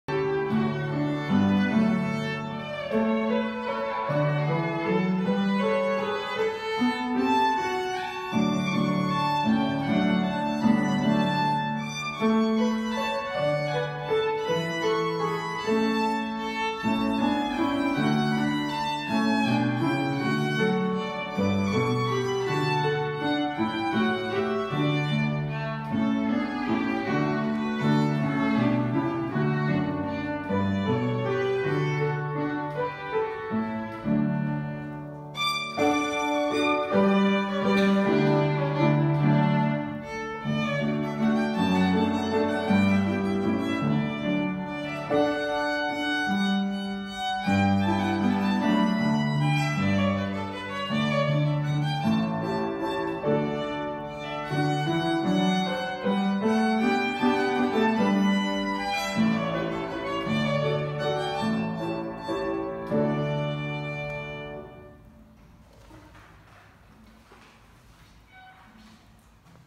Competitie - Scoala Gimnaziala de Arte nr. 3 ”Cuibul cu barza” - Bucuresti
Violin: Patricia Coteanu